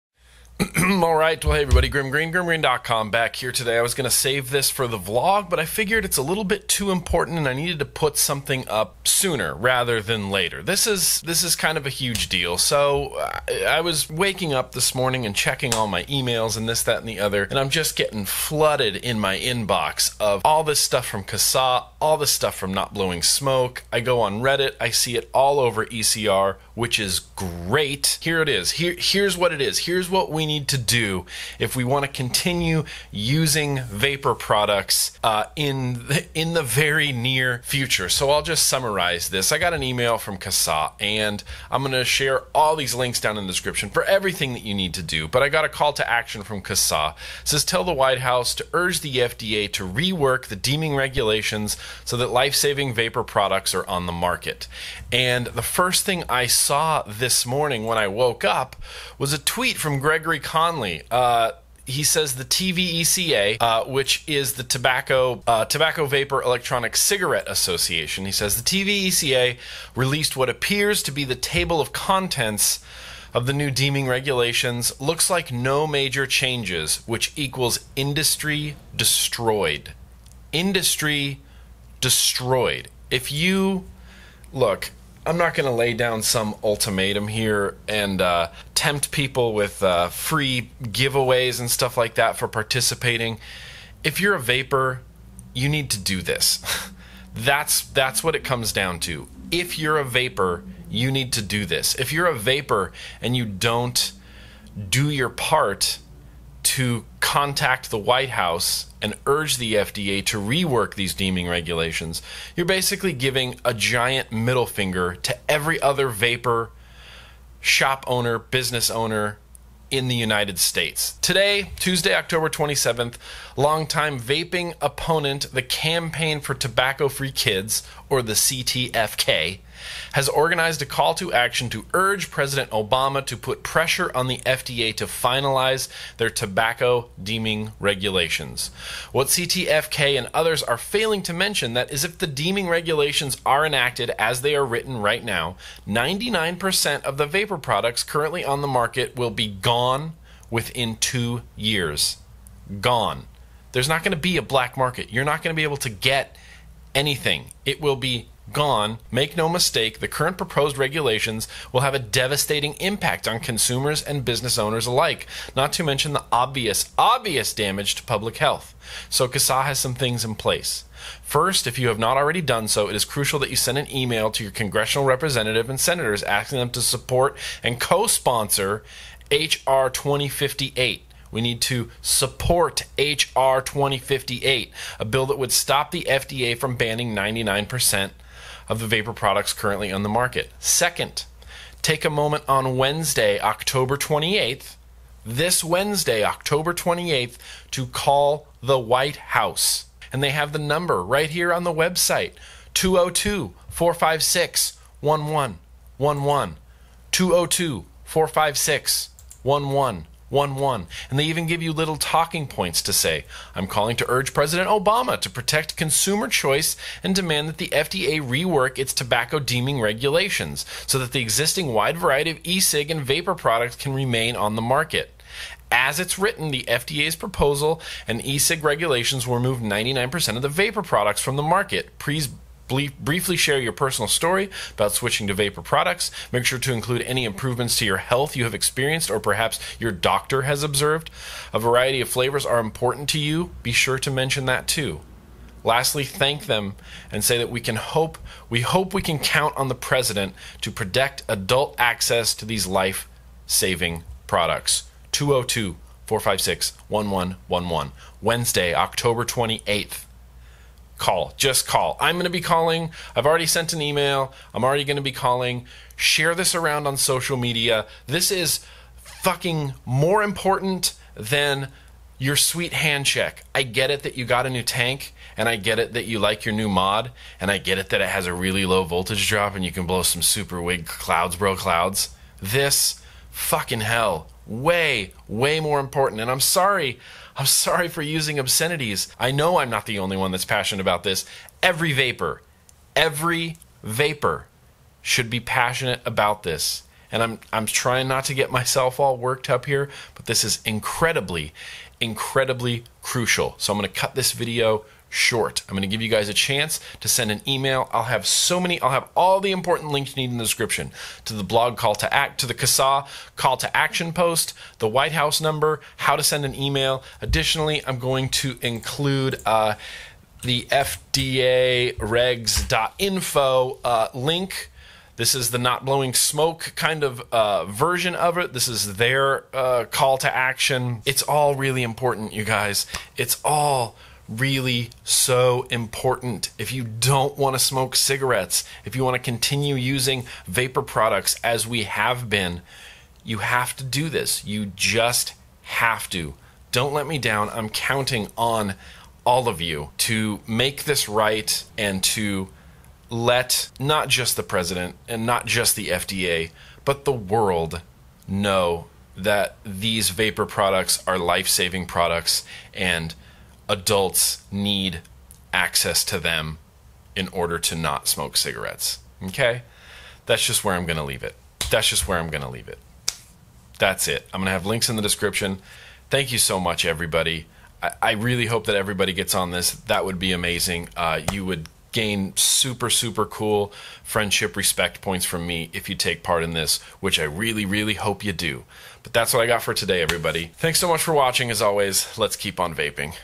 VAPING WILL BE GONE ~ Support HR2058 ~ 10/27/15
Today, Tuesday, October 27th, longtime vaping opponent the Campaign for Tobacco-Free Kids (CTFK) has organized a call-in action to urge President Obama to put pressure on the FDA to finalize their Tobacco Deeming Regulations.

What CTFK and others are failing to mention is that if the deeming regulations are enacted as written, 99.9% of the vapor products currently on the market will be gone within two to four years. Make no mistake, the current proposed regulations will have a devastating impact on consumers and businesses alike; not to mention the obvious damage to public health.

Following this Call To action is not optional, it's your duty as a vaper.  If vaping has helped you or someone you know or love in any way.  I believe in this community, and I believe we will do the right thing!